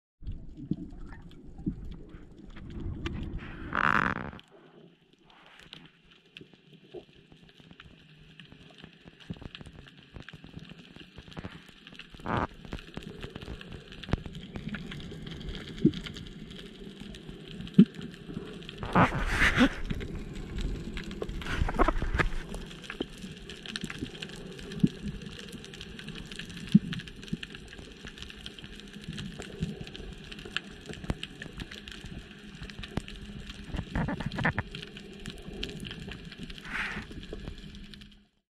UNE APNEE A RIOU JUIN 2021
La belle saison qui reprend :-)
Une petite apnée à Riou avec les copains.
Un gros mérou et de beaux corbs, ça fait toujours plaisir de voir ça ;-)